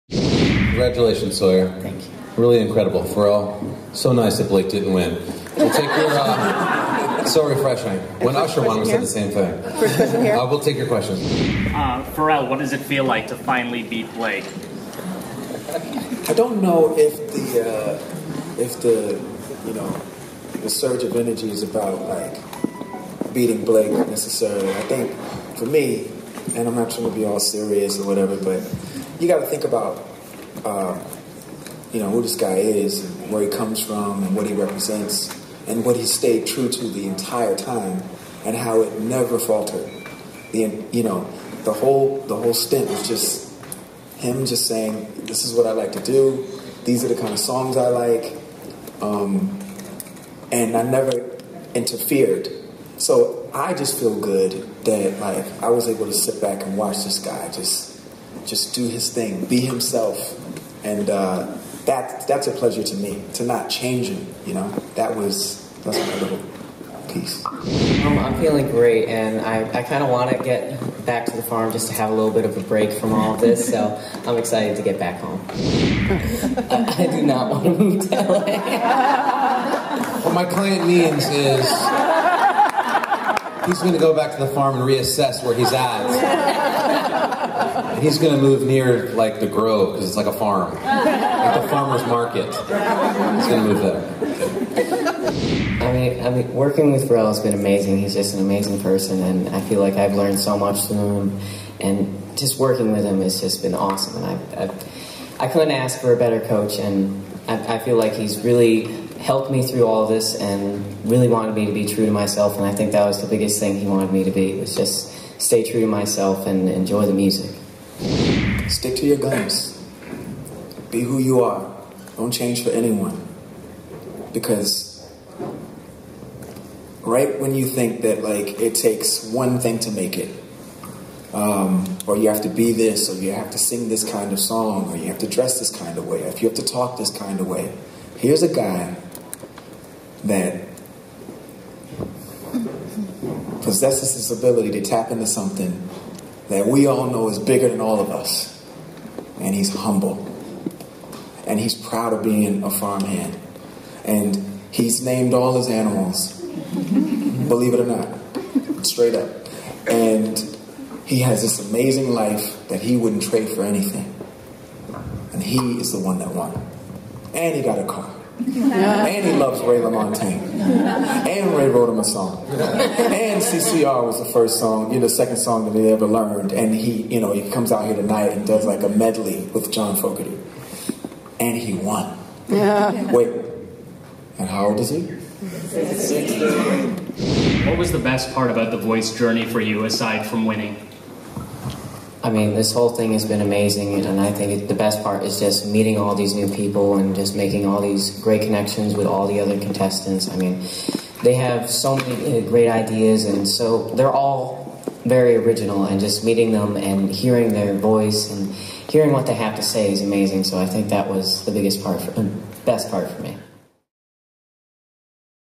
The Voice Finale Press Conference Highlights With Pharrell, Sawyer Fredericks & Carson Daly
Murtz Jaffer interviews The Voice's Pharrell and winner Sawyer Fredericks on May 19, 2015 as part of a panel with host Carson Daly.